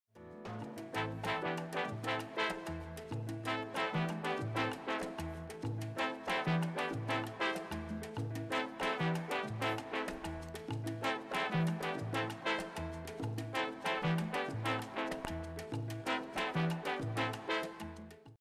Hook And Pivot Turn Fundamentals - Salsa Dance Lesson, Hector Gutierrez, Maria - guajira #1028
with Hector Gutierrez and Maria - guajira. This lesson teaches the essential footwork for right and left pivot turns and hook turns. Breakdown includes drills for mastering these two figures, timing details for both partners, plus arm-styling tips for ladies!